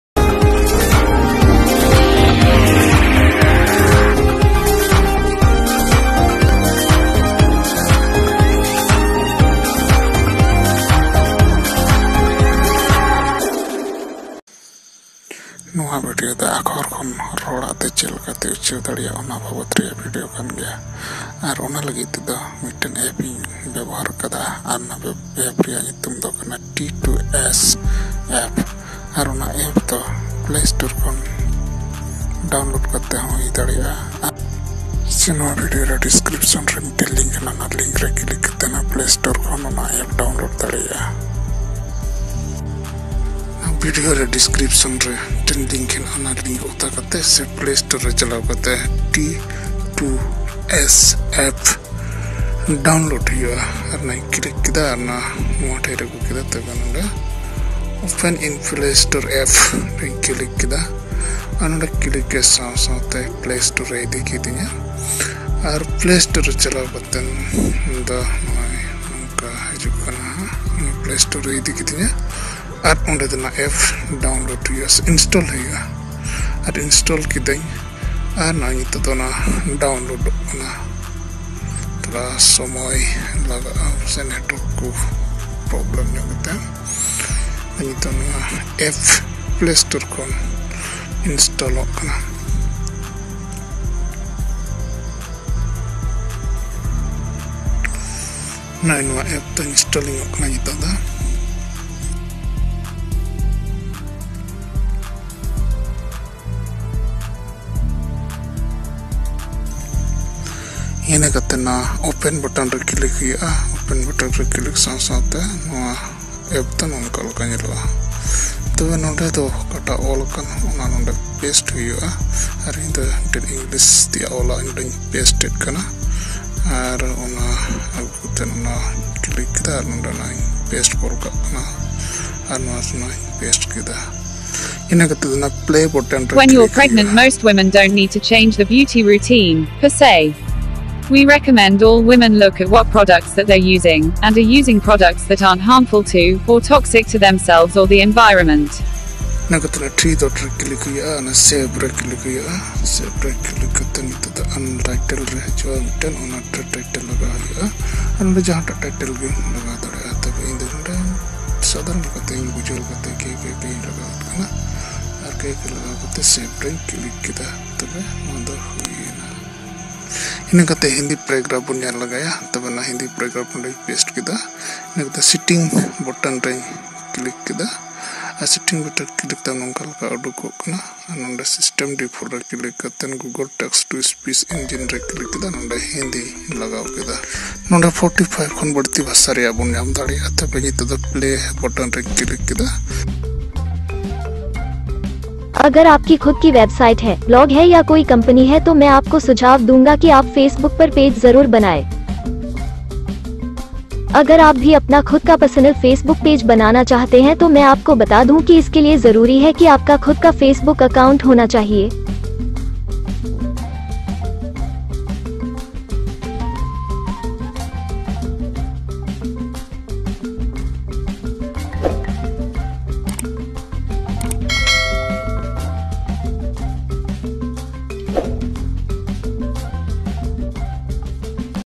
How to convert text to Voice//santali Techno Channel
This video shows, how to convert text to Voice by using android app. The language of this video is santali.